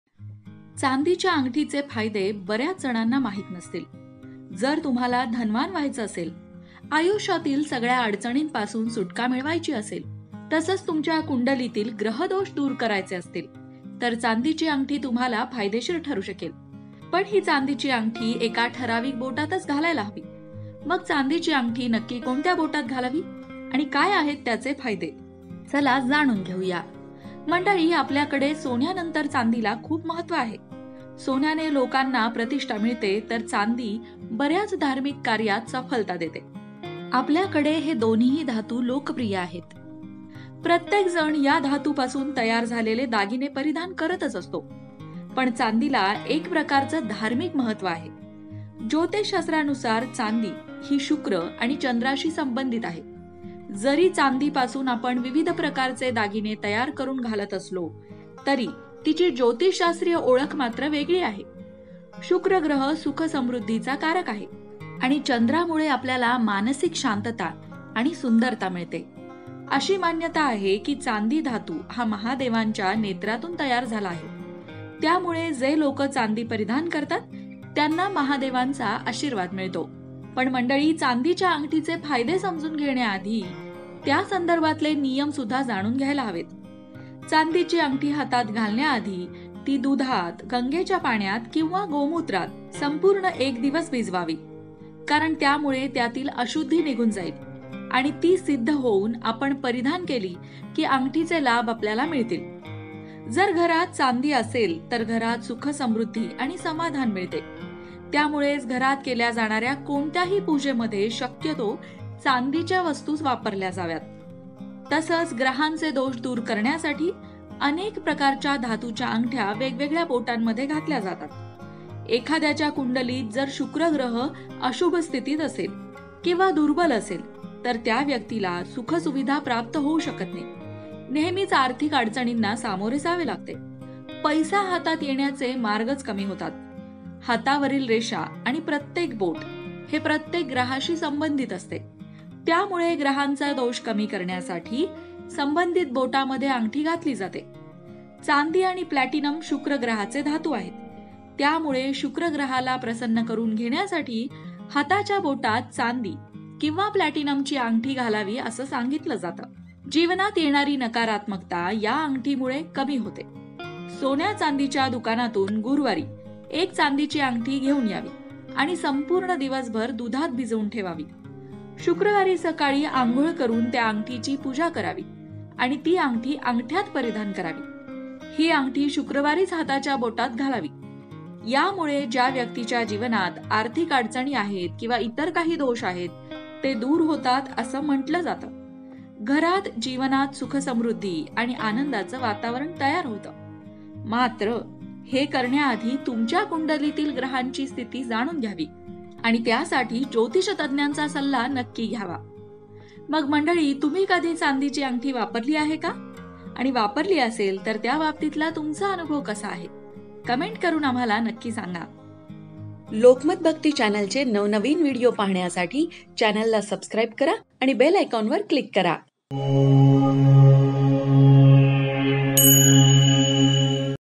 चांदीची अंगठी घातल्याने होणारे फायदे | Benefits of wearing a Silver Ring | Silver Ring Information
आपल्या हातामधील बोटांमध्ये अंगठी घालण्याची आवड प्रत्येकालाच असते असे नाही. पण आपण जर चांदीची अंगठी बोटांमध्ये घातली तर त्याचे काय फायदे होतील? त्याबद्दल जर तुम्हाला अचूक माहिती जाणून घ्यायची असेल तर हा व्हिडीओ नक्की बघा -